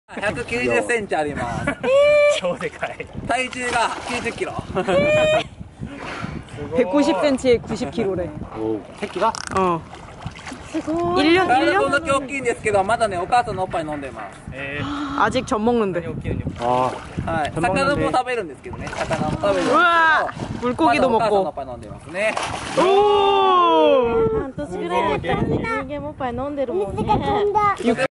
돌고래 보러 오키나와에 가자!!!(イルカを見に沖縄へ行こう!！)
돌고래체험/ 오키나와 북부 여행지/  알려지지 않은 핫플레이스/ 돌고래를 저렴한 비용으로 눈앞에서 관람
イルカ体験／沖縄の北部観光地／よく知られてないホットスポット／眼の前でイルカ観覧